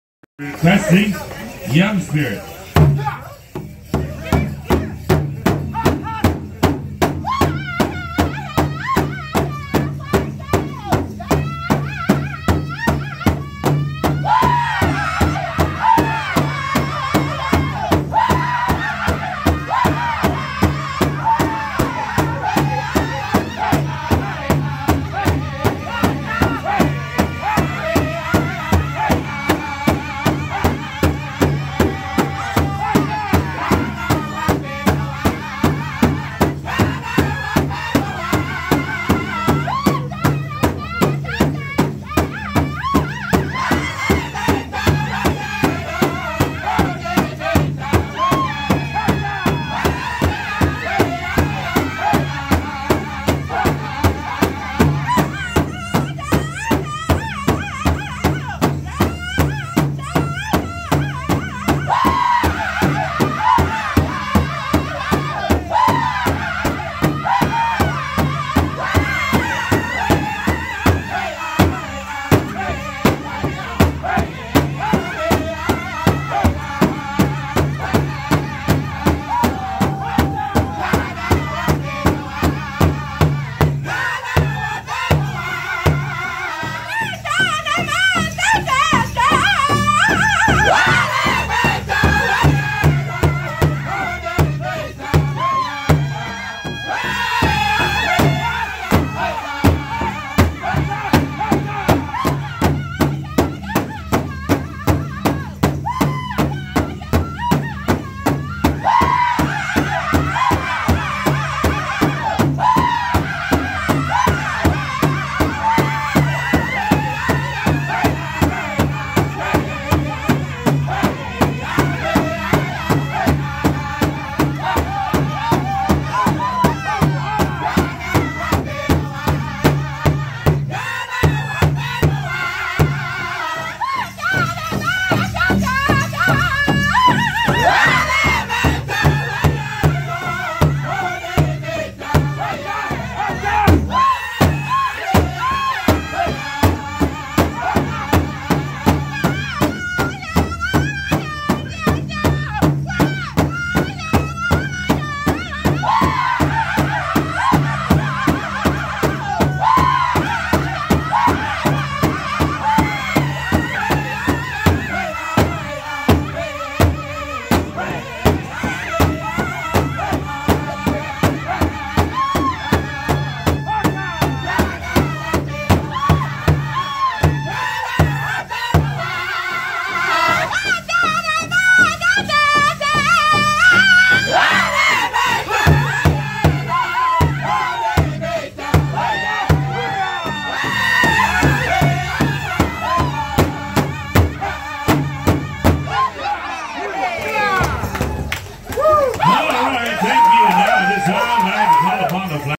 Young Spirit @ Wildhorse 2017
Young Spirit (host drum) singing for Men's Traditional at Wildhorse 2017